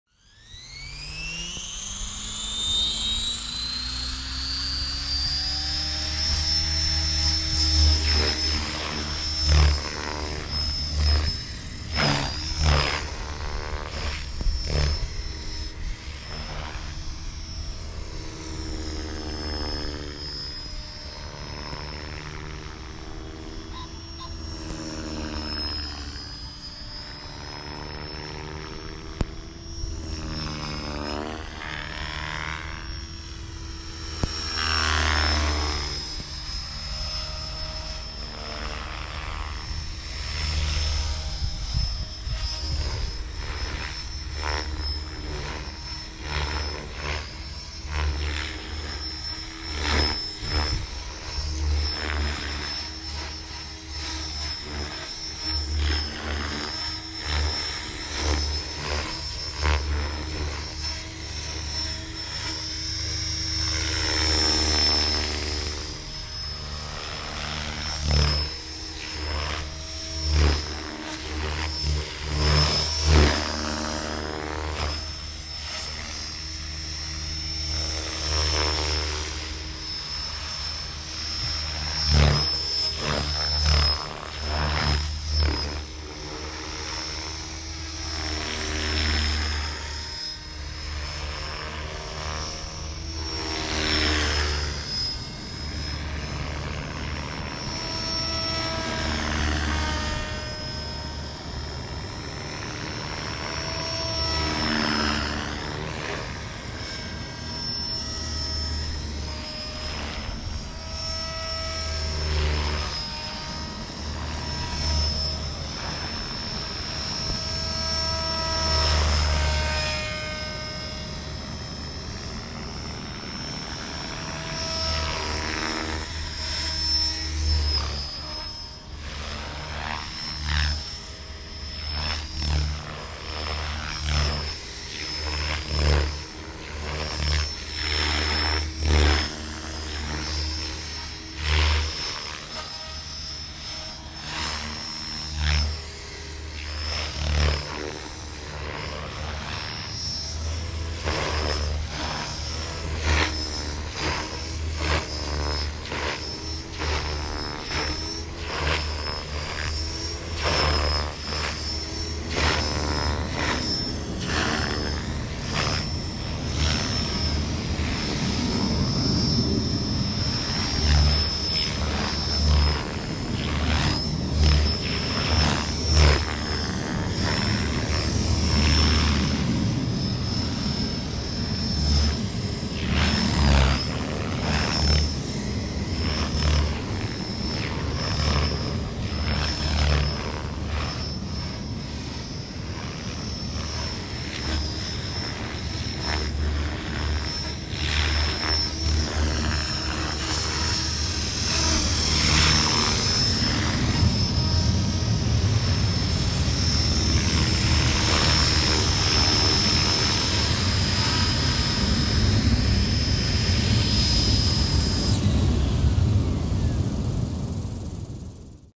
2023.05.23 Beam Archon SE Flight
* * * * * * Share specs* * * * * * *
Kit : BEAM Archon SE (500Size)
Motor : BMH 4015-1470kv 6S
ESC : Scorpion Tribunus 06-80A 6S
Cyclic Servo : [GDW] DS590MG Digital MIni Size Servo *3
Tail Servo : [Align] DS535HV Digital Middle Size Tail Servo *1
BEC : [HOBBYWING] UBEC 10A - 7.4V
Gyro : Mikado VBar NEO
Battery : HRB 6S 3300mAh 60C